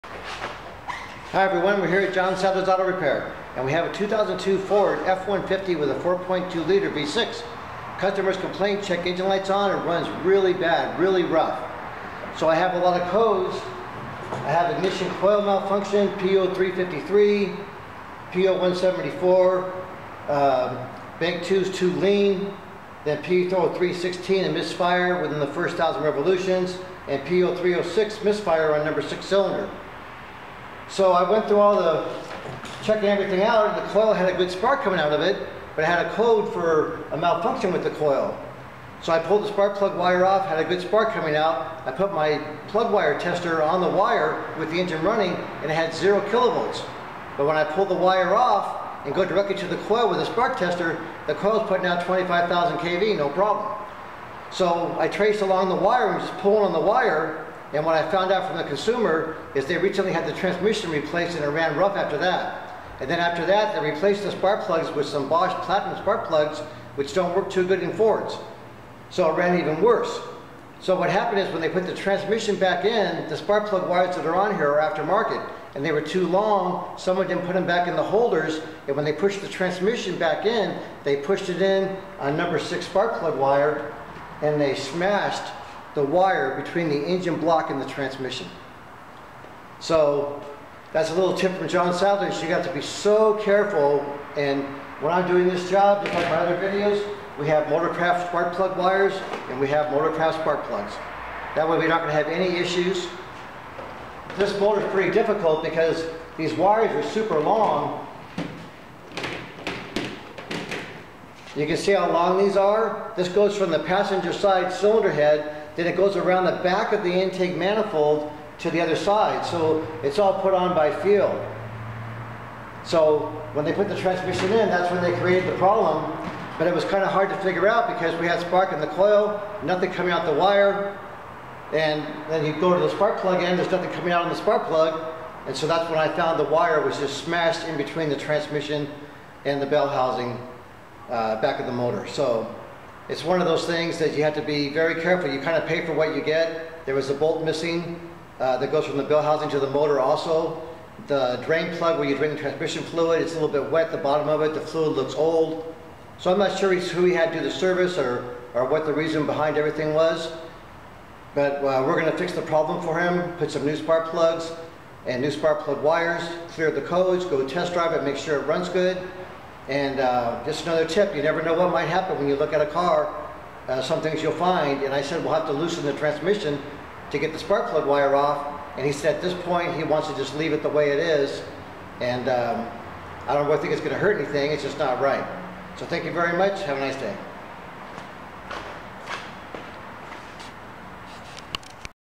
2002 Ford F-150 4.2L --Runs rough after the Transmission was Removed
HI Everyone: This was an interesting job, the customer informed me his engine is running rough. He stated another shop replaced his spark plugs with Bosch Platinum plugs and made no difference.
After asking numerous questions the customer informed me that it happened after the transmission was removed to install a new clutch. Has codes for ignition coil malfunction, misfires within the first 1,000 revolutions and misfire on number 6 cylinder. The coil tested good and the plug wires tested bad, when I pulled on the plug wire to remove it I found it was smashed in between the transmission and engine block.